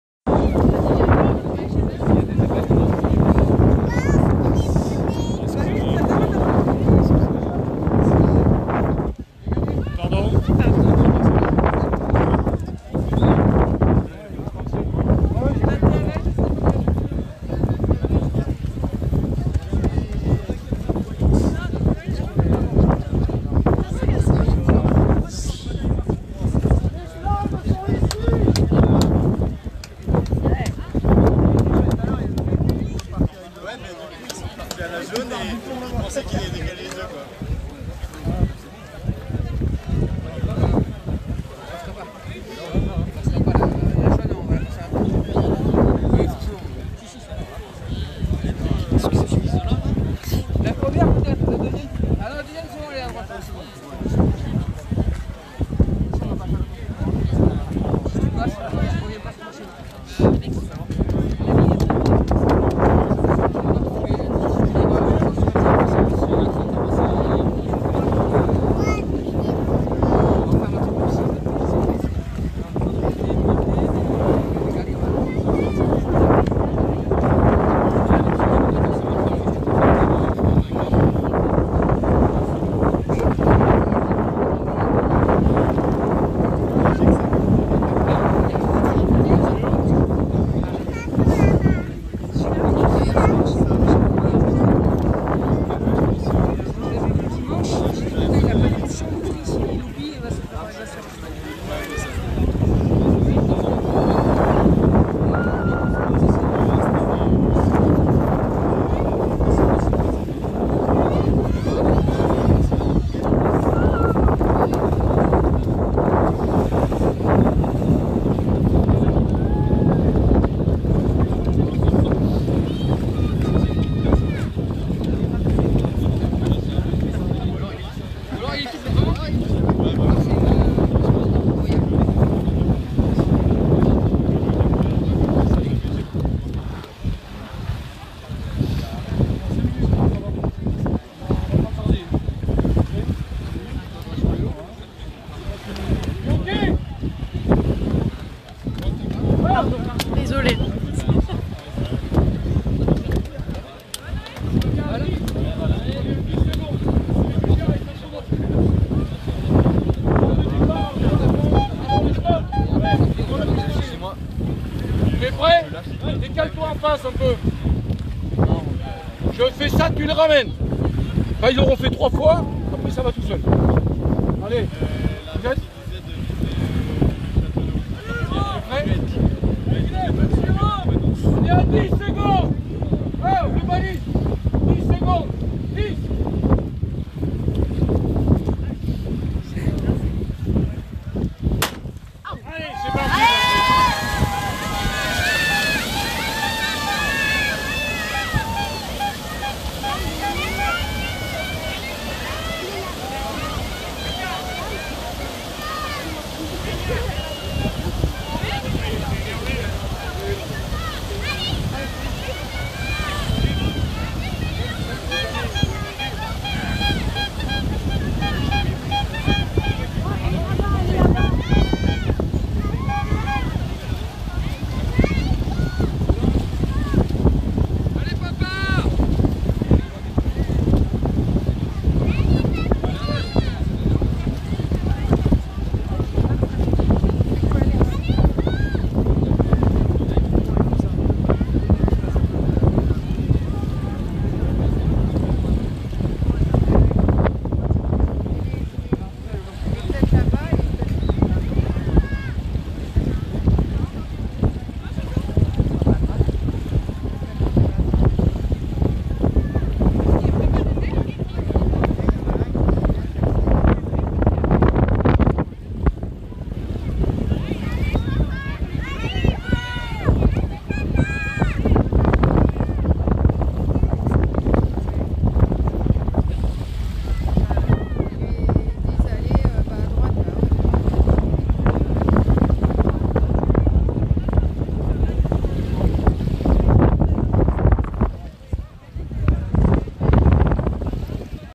Nouveau départ pour le triathlon d'Arcachon
Le triathlon d'Arcachon faisait son retour samedi plage des arbousiers après deux ans d'interruptions liées au Covid. Les participants comme le soleil étaient au rendez-vous !